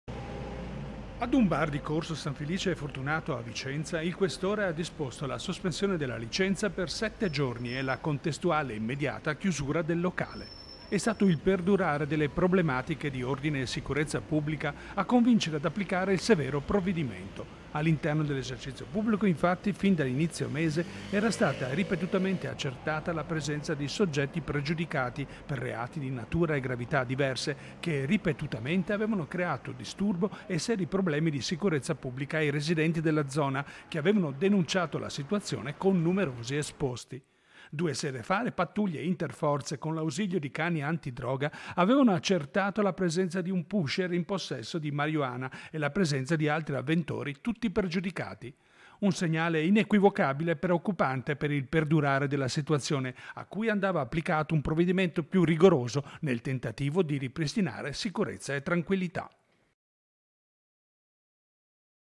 LICENZA SOSPESA AD UN BAR PER 7 GIORNI: TROPPI PREGIUDICATI | 27/08/2022
TG Vicenza 27/08/2022 - VICENZA - Troppi problemi di ordine pubblico.  Un bar di Vicenza è stato chiuso dalla Questura.  || Ad un bar di corso San Felice e Fortunato a Vicenza il questore ha disposto la sospensione della licenza per 7 giorni e la contestuale, immediata chiusura del locale. E’ stato il perdurare delle problematiche di ordine e sicurezza pubblica a convincere ad applicare il severo provvedimento. All’interno dell’esercizio pubblico infatti fin da inizio del mese era stata ripetutamente accertata la presenza di soggetti pregiudicati per reati di natura e gravità diverse che ripetutamente avevano creato disturbo e seri problemi di sicurezza pubblica ai residenti della zona che avevano denunciato la situazione con numerosi esposti. Due sere fa le pattuglie interforze con l’ausilio di cani antidroga avevano accertato nel la presenza di un pusher in possesso di marijuana e la presenza di altri avventori tutti pregiudicati. Un segnale inequivocabile preoccupante per il perdurare della situazione a cui andava applicato un provvedimento più rigoroso nel tentativo di ripristinare sicurezza e tranquillità. (Servizio di Daniele Garbin) - Segui Rete Veneta anche sul digitale terrestre! Visita il sito